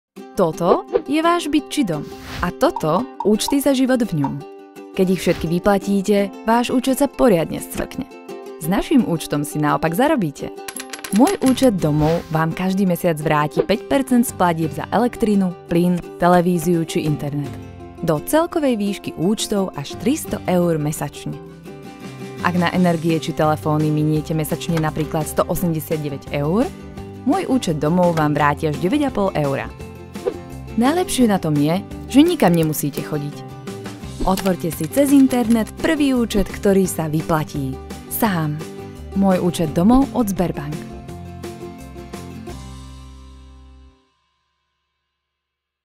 Sberbank Slovensko | môj ÚČET DOMOV | short
S novým online účtom od Sberbank pôjdu ceny energií dole.
Otvorte si ho z pohodlia vášho domova a ušetrite mesačne až 5% z účtov za elektrinu, plyn, telefón, ale i TV či internet. Môj účet domov sa vám skrátka vyplatí. A dokonca sám.